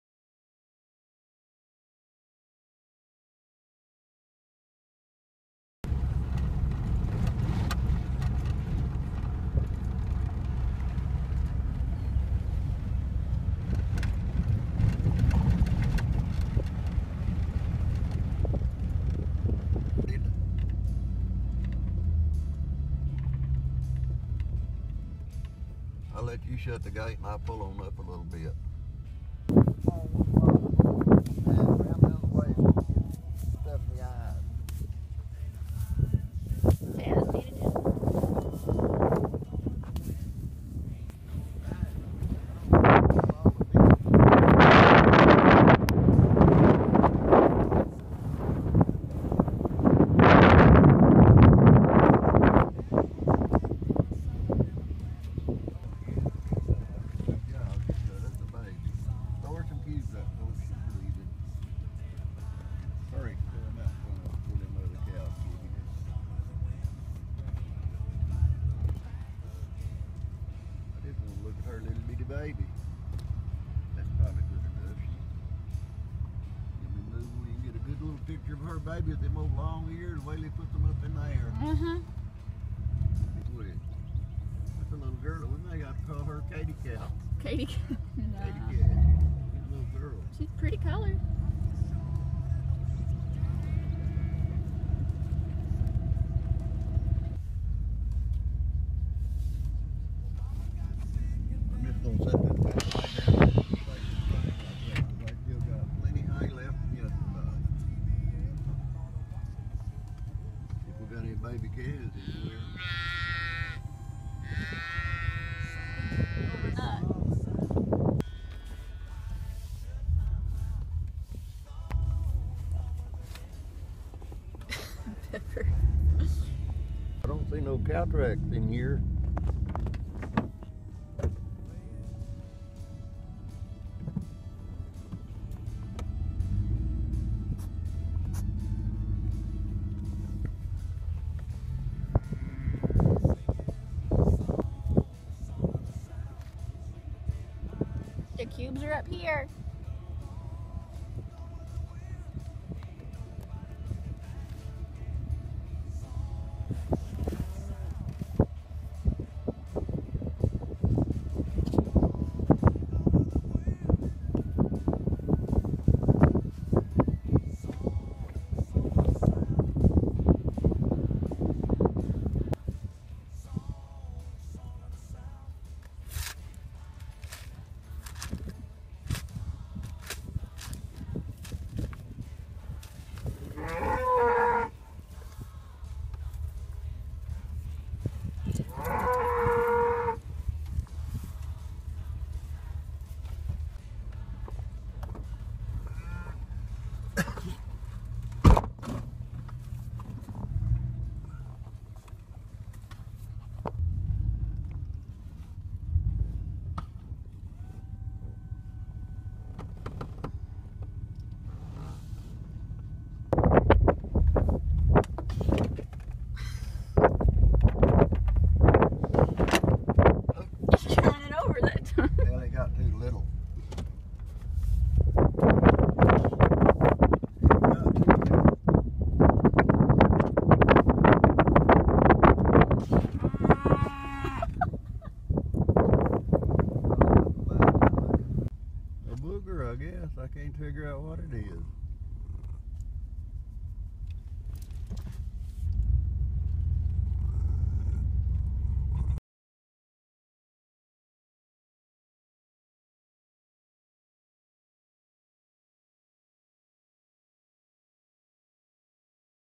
Papaw Jack's Cattle (Feb. 2017)
Decided to film one day in February, while spending the day with Papaw Jack. It's Cow business every day for him, come rain, freeze, sweat, or shine. Hay feeding season was coming to a close as new born calf season was starting to kick off. The most important part for him, seems to be keeping up with the dogs and wondering where Pepper went.